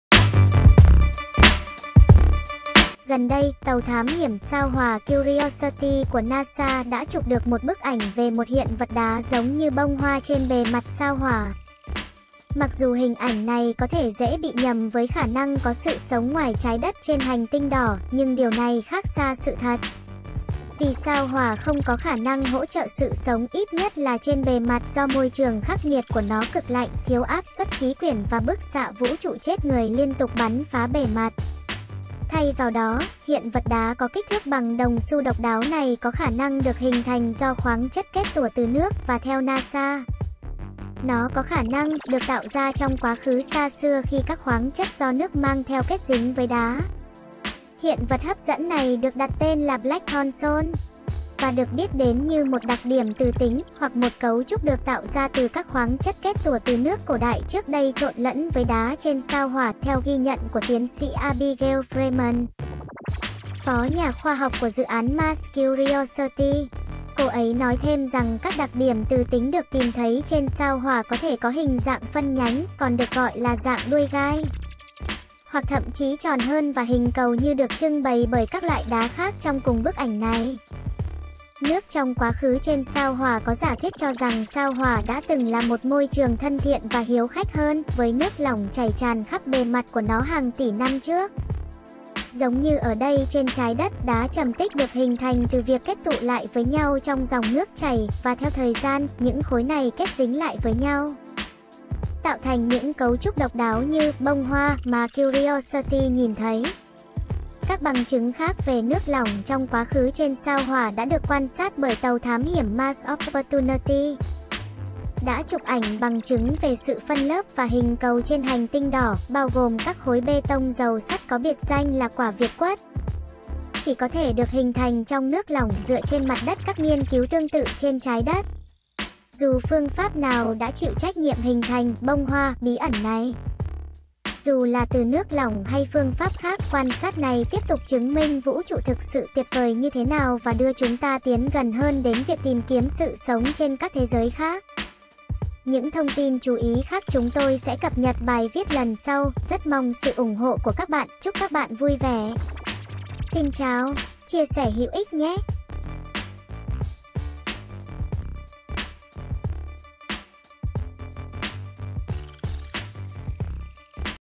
Chúng tôi đã không tìm thấy một bông hoa trên sao Hỏa
Gần đây, tàu thám hiểm sao Hỏa Curiosity của NASA đã chụp được một bức ảnh về một hiện vật đá giống như bông hoa trên bề mặt sao Hỏa.
Mặc dù hình ảnh này có thể dễ bị nhầm với khả năng có sự sống ngoài Trái đất trên Hành tinh Đỏ, nhưng điều này khác xa sự thật, vì sao Hỏa không có khả năng hỗ trợ sự sống (ít nhất là trên bề mặt) do môi trường khắc nghiệt của nó - cực lạnh, thiếu áp suất khí quyển và bức xạ vũ trụ chết người liên tục bắn phá bề mặt.
Thay vào đó, hiện vật đá có kích thước bằng đồng xu độc đáo này có khả năng được hình thành do khoáng chất kết tủa từ nước , và theo NASA , nó có khả năng "được tạo ra trong quá khứ xa xưa khi các khoáng chất do nước mang theo kết dính với đá."

Hiện vật hấp dẫn này được đặt tên là Blackthorn Salt , và được biết đến như một đặc điểm từ tính, hoặc một cấu trúc được tạo ra từ các khoáng chất kết tủa từ nước cổ đại trước đây trộn lẫn với đá trên sao Hỏa, theo ghi nhận của Tiến sĩ Abigail Fraeman, Phó nhà khoa học của dự án Mars Curiosity. Cô ấy nói thêm rằng các đặc điểm từ tính được tìm thấy trên sao Hỏa có thể có hình dạng phân nhánh, còn được gọi là dạng đuôi gai, hoặc thậm chí tròn hơn và hình cầu như được trưng bày bởi các loại đá khác trong cùng bức ảnh này.

Nước trong quá khứ trên sao Hỏa

Có giả thuyết cho rằng sao Hỏa đã từng là một môi trường thân thiện và hiếu khách hơn, với nước lỏng chảy tràn khắp bề mặt của nó hàng tỷ năm trước. Giống như ở đây trên Trái đất, đá trầm tích được hình thành từ việc kết tụ lại với nhau trong dòng nước chảy, và theo thời gian, những khối này kết dính lại với nhau, tạo thành những cấu trúc độc đáo như “bông hoa” mà Curiosity nhìn thấy.

Các bằng chứng khác về nước lỏng trong quá khứ trên sao Hỏa đã được quan sát bởi tàu thám hiểm Mars Opportunity, đã chụp ảnh bằng chứng về sự phân lớp và hình cầu trên Hành tinh Đỏ, bao gồm các khối bê tông giàu sắt có biệt danh là “quả việt quất” , chỉ có thể được hình thành trong nước lỏng dựa trên mặt đất các nghiên cứu tương tự trên Trái đất.

Dù phương pháp nào đã chịu trách nhiệm hình thành “bông hoa” bí ẩn này, dù là từ nước lỏng hay phương pháp khác, quan sát này tiếp tục chứng minh vũ trụ thực sự tuyệt vời như thế nào và đưa chúng ta tiến gần hơn đến việc tìm kiếm sự sống trên các thế giới khác.